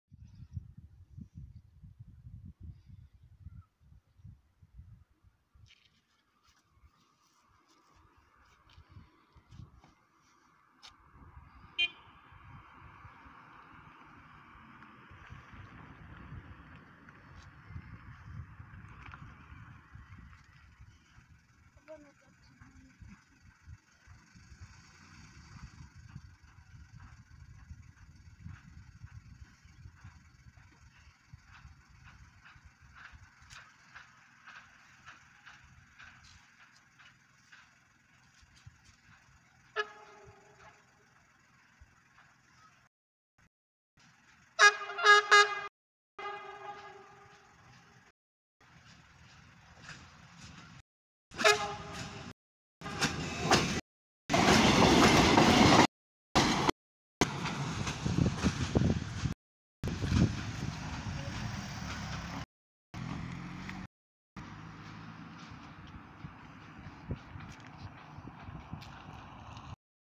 Vonatot kürtöltünk 2020.08.24. Hétfő
33523 S780 as Személyvonat (Balassagyarmat-Aszód) 2 Darab bzmot 117 341 Második 117 372